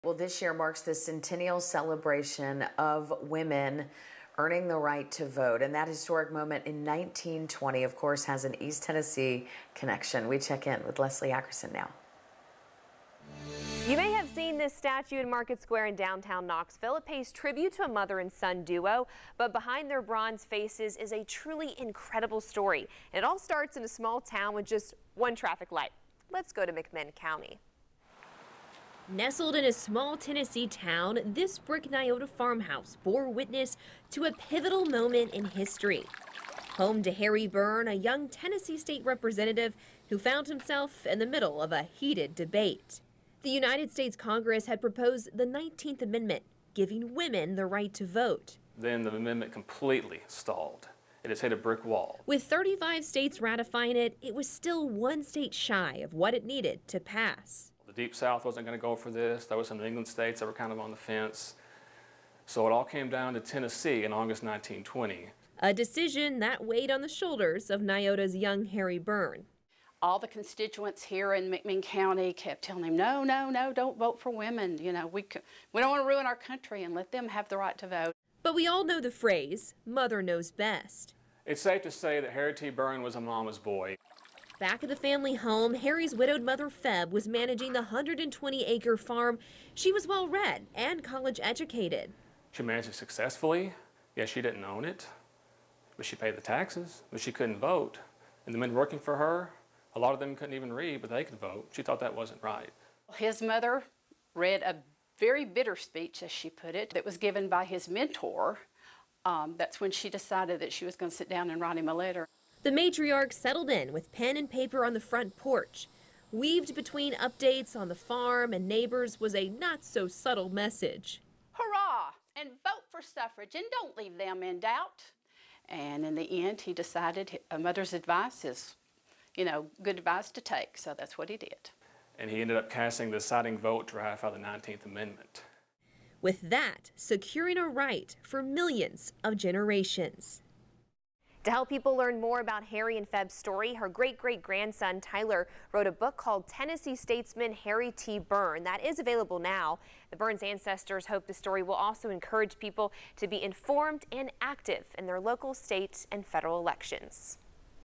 Hathburn: The house that witnessed history for women's suffrage, 19th Amendment
100 years ago, on the front porch of their home, Febb Burn wrote a letter to her son, State Rep. Harry T. Burn, and helped ratify the 19th Amendment, securing votes for women. This home still stands in McMinn County, Tennessee, and Febb and Harry's descendants use their stories to keep their legacy alive.